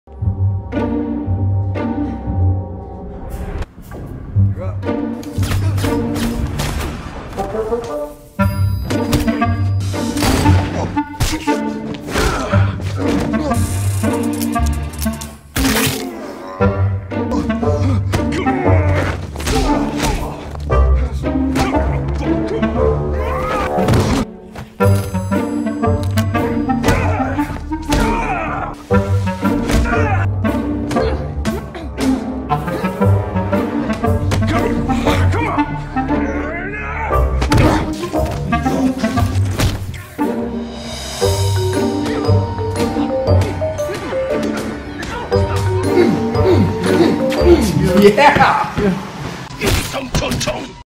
The Heel Reel or (A Fight Reel for a Classically Trained, Ethnically Ambiguous Punching Bag)
Going over my Film and TV career, I realized I had a lot of footage of me getting my ass kicked.  I decided to cut a comedic montage of the greatest hits. 🤣🥊